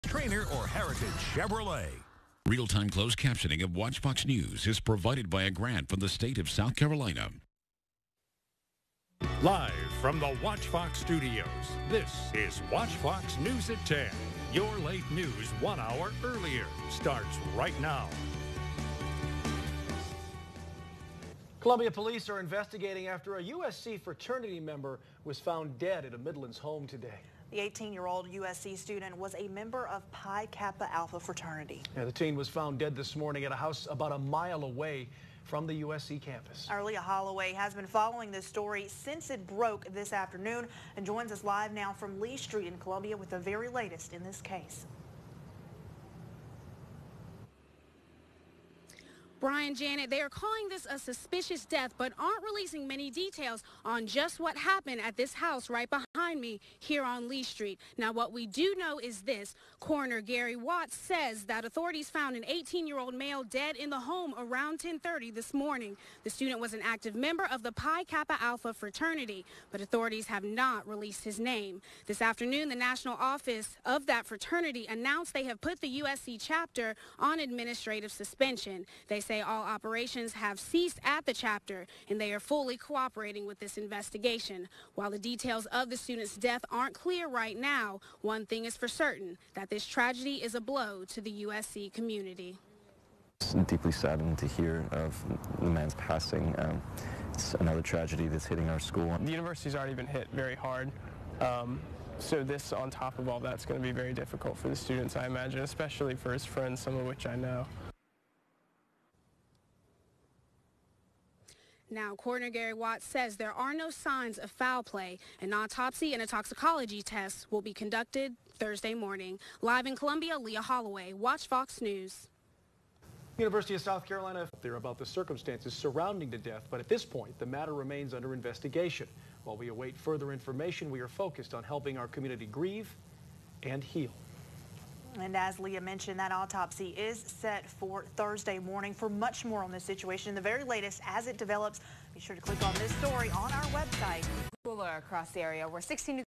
USC STUDENT DEATH
THE 18 YEAR OLD TEEN WAS FOUND DEAD THIS MORNING AT A HOUSE ABOUT A MILE AWAY FROM THE USC CAMPUS.